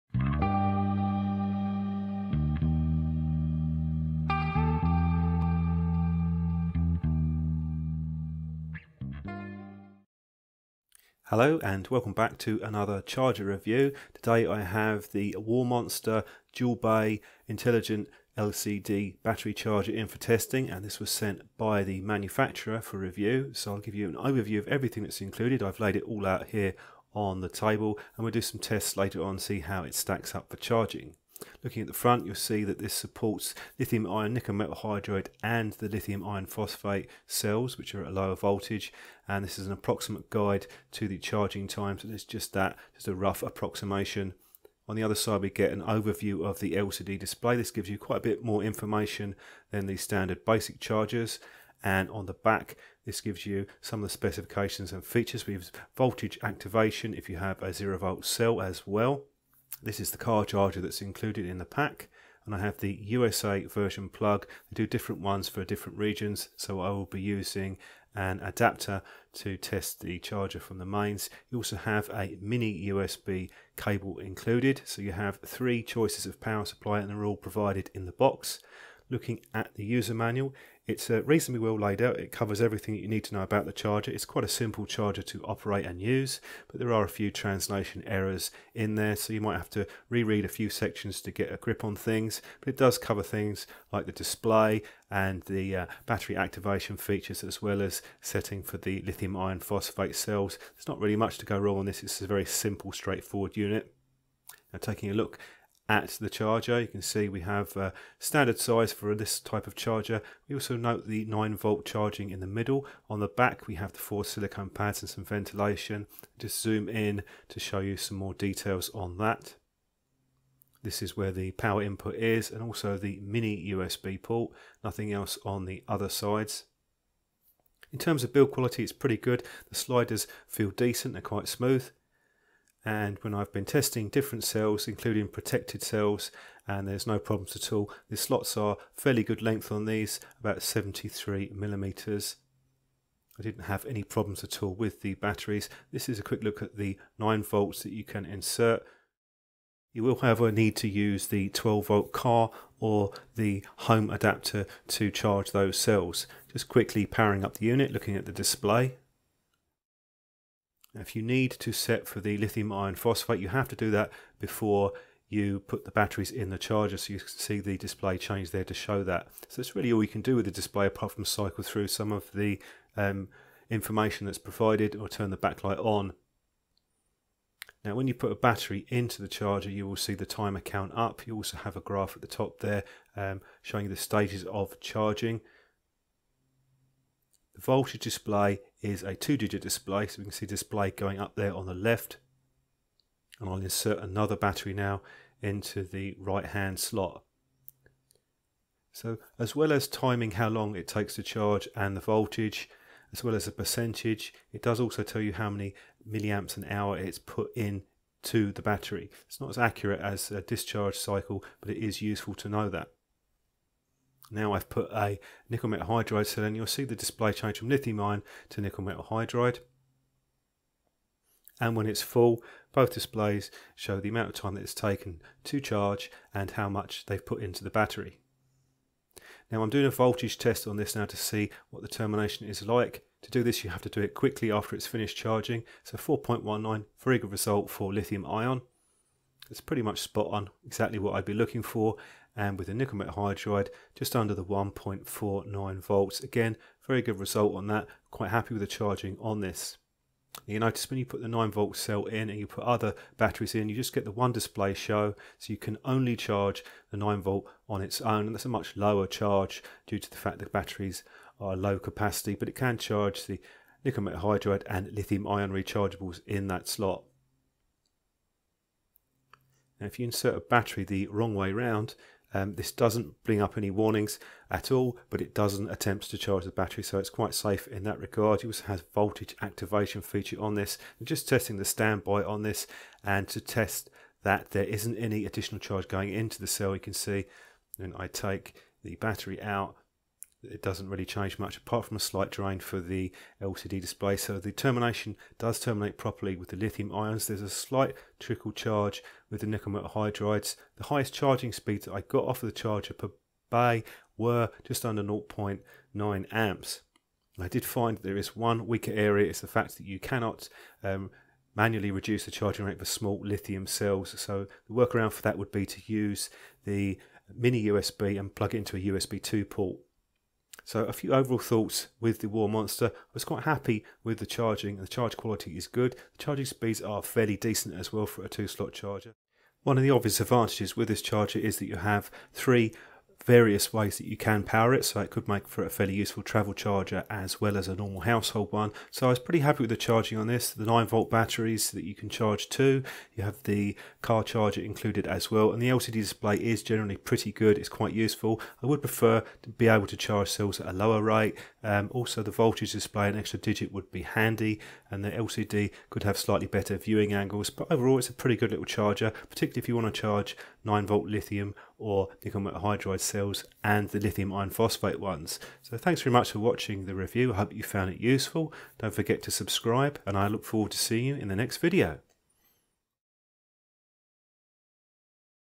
Warmonster H2 Universal Smart Charger - Review
My hands on review of the 2 slot charger from Warmonster. This model features charging for Ni-MH, Li-ion and 9 volt batteries and comes with a mains adapter, car charger and mini USB lead.

I test the charger to see how it fares compared to other similar models. Also a comparison with the Xtar VC2 Plus Master.

Sample supplied by the manufacturer for independent review

Details from the maker:
Note : 9v battery just only can be charged by 12V plug.

Spec:
DC IN : 5V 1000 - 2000mA / 12V 1000mA
Output : 1000mA * 2 @ Li-ion 3.7v Battery / @ Ni-MH 1.2v Battery /@ LiFePO4 3.2v Battery / 8.4v,450mA (9V Li-ion) 300mA(9V Ni-MH)
Charge mode : CC/CV (Li-ion Battery / LiFePO4 Battery ) / DELTA V (Ni-MH Battery )

Features:
1.Press the button select LiFePO4 or NiMH/Li-ion batteries ;Automatic 3.7v/1.2v battery cell voltage and polarity detection.
2.Each of the two battery slots monitors and charges independently.
3.Intelligent circuitry detects the battery type and status before entering automatic charge mode (CC, CV, dV/dt).
4.Automatically detects battery power status and selects the appropriate voltage and charge mode (with the exception of LiFePO4 batteries which require manual selection).
5.Mix charge the battery , Reverse protection , Precision end-of-charge voltage detection. Over charging timeout protection.
6.Auto active function for Lithium battery cell open (0 Voltage).
7.Auto alert indication for worse battery cell and bad battery cell.

Package:
1 * Charger
1 * Car Charger
1 * US-Plug
1 * USB Cable